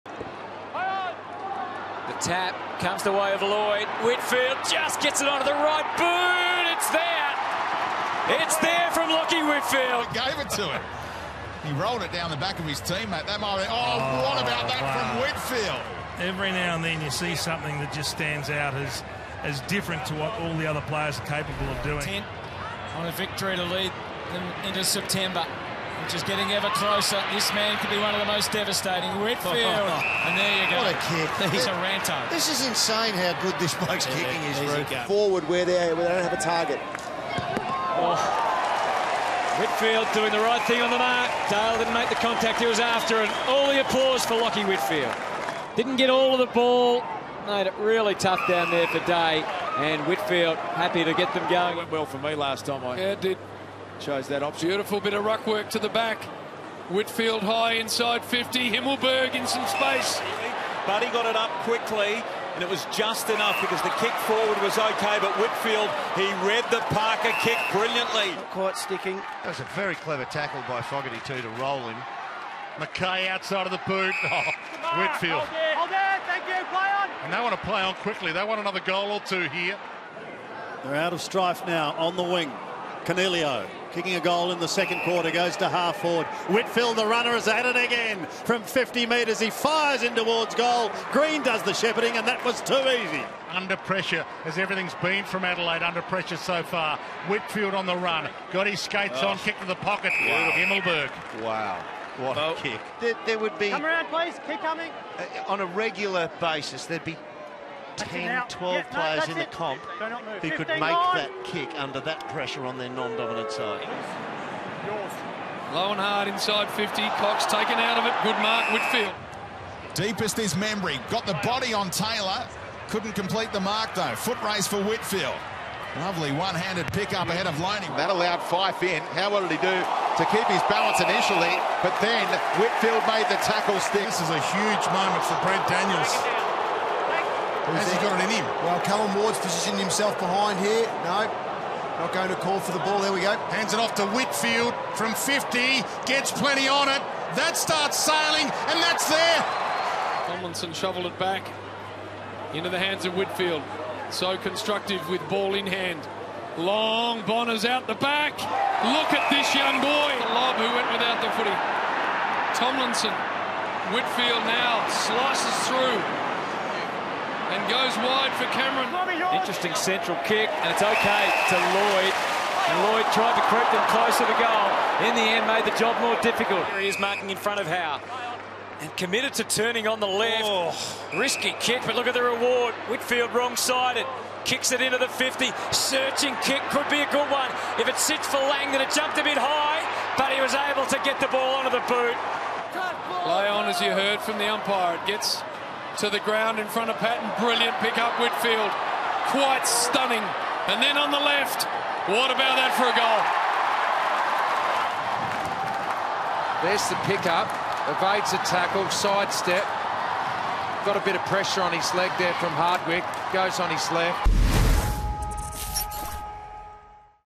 The best of Lachie Whitfield in 2018 | Club Champion Series | AFL
Enjoy highlights from GWS best and fairest Lachie Whitfield.

Kevin Sheedy Medal
How they vote: Leon Cameron and his assistants Brad Miller, Lenny Hayes and Mark McVeigh vote on the award. Up to four votes can be awarded to a player by each coach every round, meaning each player is able to receive a maximum of 16 votes per round.

Top 5
1. Lachie Whitfield 175
2. Callan Ward 164
3. Phil Davis 156
4. Stephen Coniglio 154
5. Nick Haynes 151